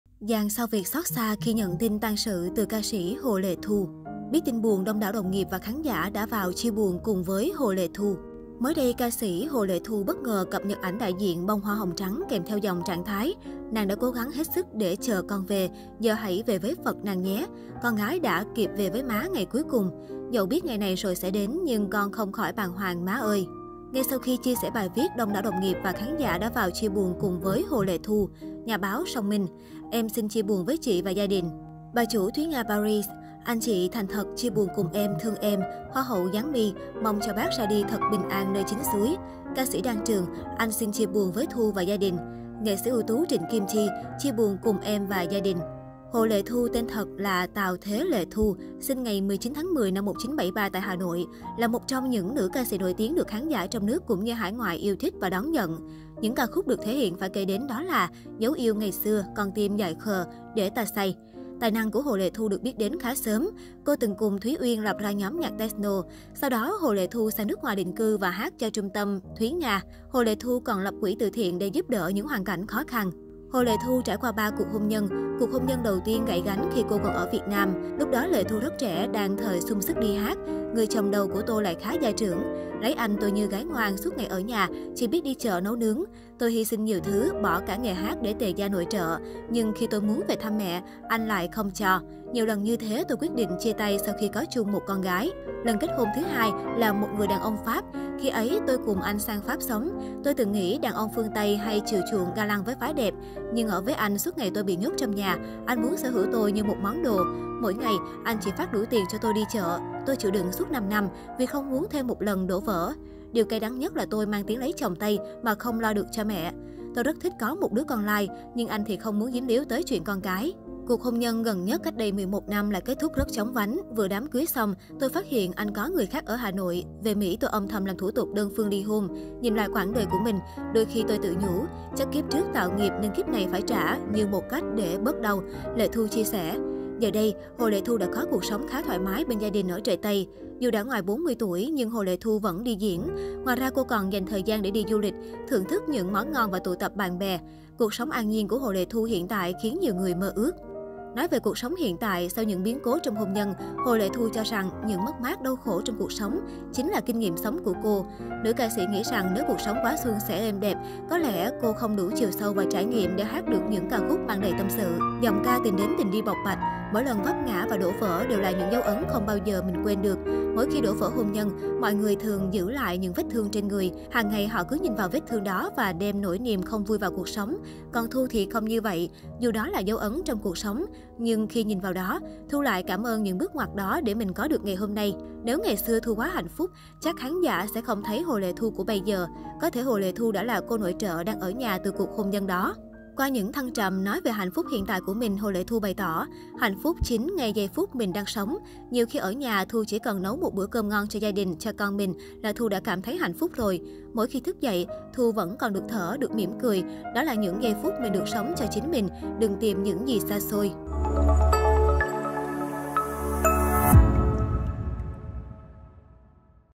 Dàn sao Việt xót xa khi nhận tin tang sự từ ca sĩ Hồ Lệ Thu
Biết tin buồn, đông đảo đồng nghiệp và khán giả đã vào chia buồn cùng với Hồ Lệ Thu.
Mới đây, ca sĩ Hồ Lệ Thu bất ngờ cập nhật ảnh đại diện bông hoa hồng trắng kèm dòng trạng thái : "Nàng đã cố gắng hết sức để chờ con về, giờ hãy về với Phật nàng nhé. Con gái đã kịp về với má ngày cuối cùng. Dẫu biết ngày này rồi sẽ đến nhưng con không khỏi bàng hoàng má ơi."